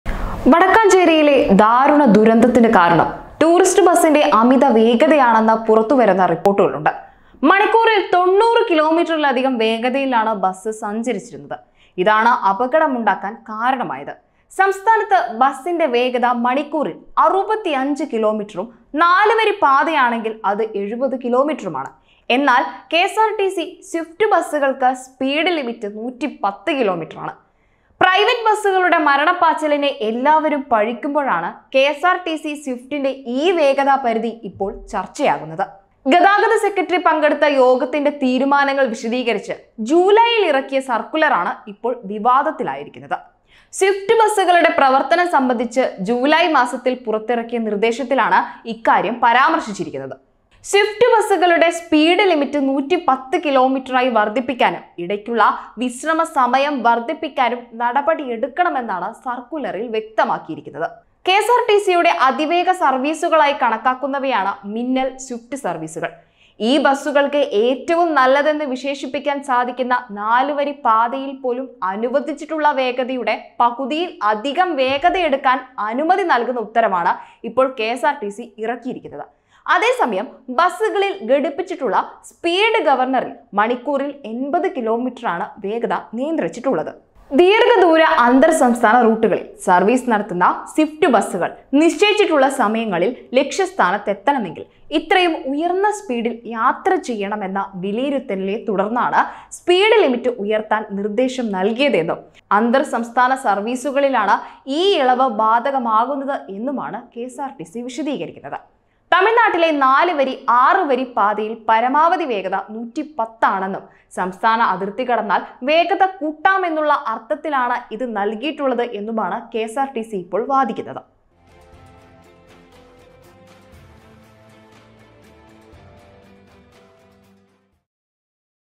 കെഎസ്ആര്‍ടിസി എന്തുമാകാമോ |Ksrtc
വടക്കഞ്ചേരിയിലെ ദാരുണ ദുരന്തത്തിന് കാരണം ടൂറിസ്റ്റ് ബസിന്റെ അമിത വേഗതയാണെന്നാണ് പുറത്ത് വരുന്ന റിപ്പോര്‍ട്ടുകള്‍. മണിക്കൂറില്‍ 90 കിലോമീറ്ററിലധികം വേഗതയിലാണ് ബസ് സഞ്ചരിച്ചിരുന്നത്. ഇതാണ് അപകടം ഉണ്ടാക്കാന്‍ കാരണമായത്. സംസ്ഥാനത്ത് ബസിന്റെ വേഗത മണിക്കൂരല്‍ 65 കിലോമീറ്ററും നാല് വരി പാതകളിലാണെങ്കില്‍ അത് 70 കിലോമീറ്ററുമാണ്.